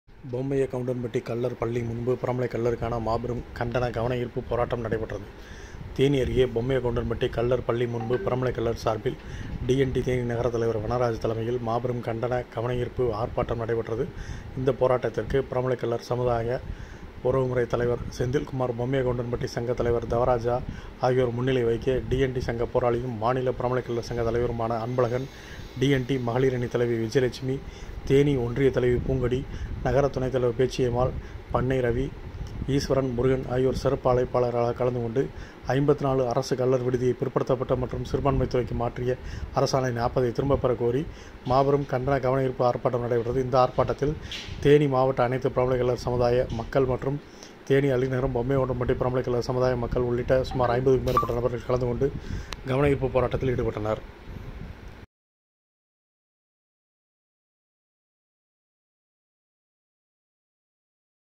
பொம்மையகவுண்டன்பட்டி கள்ளர் பள்ளி முன் பிரமலை கள்ளருக்கான மாபெரும் கண்டன கவனயீர்ப்பு போராட்டம்
தேனி அருகே பொம்மைய கவுண்டன்பட்டி கள்ளர் பள்ளி முன்பு பிரமலைக் கள்ளர்கள் சார்பில் DNT தேனி நகர தலைவர் வனராஜ் தலைமையில், மாபெரும் கண்டன கவனயீர்ப்பு ஆர்ப்பாட்டம் நடைபெற்றது. இந்த ஆர்ப்பாட்டத்திற்கு பிரமலை கள்ளர் சமூக உறவுமுறை தலைவர் செந்தில்குமார் பொம்மைய கவுண்டன்பட்டி சங்கத்தலைவர் தவராஜா ஆகியோர் முன்னிலை வகிக்க, DNT சங்க போராளியும் மாநில பிரமலைக்கள்ளர் சங்க தலைவருமான அன்பழகன், DNT மகளிர் அணி தலைவி விஜயலட்சுமி தேனி ஒன்றிய தலைவி பூங்கொடி நகர துணைத்தலைவர் பேச்சியம்மாள் பண்ணை ரவி ஈஸ்வரன் முருகன் ஆகியோர் சிறப்பு அழைப்பாளர்களாக கலந்து கொண்டு, 54 அரசு கள்ளர் விடுதியை பிற்படுத்தப்பட்ட மற்றும் சிறுபான்மை துறைக்கு மாற்றிய அரசாணை 40 ஐ திரும்பப் பெறக்கோரி மாபெரும் கண்டன கவனயீர்ப்பு ஆர்ப்பாட்டம் நடைபெற்றது இந்த ஆர்ப்பாட்டத்தில் தேனி மாவட்ட அனைத்து பிரமலை கள்ளர் சமுதாய மக்கள் மற்றும் தேனி அல்லிநகரம் பொம்மயகவுண்டன்பட்டி பிரமலை கள்ளர் சமுதாய மக்கள் உள்ளிட்ட சுமார் 50க்கும் மேற்பட்ட நபர்கள் கலந்து கொண்டு கவன ஈர்ப்பு ஆர்ப்பாட்டத்தில் ஈடுபட்டனர்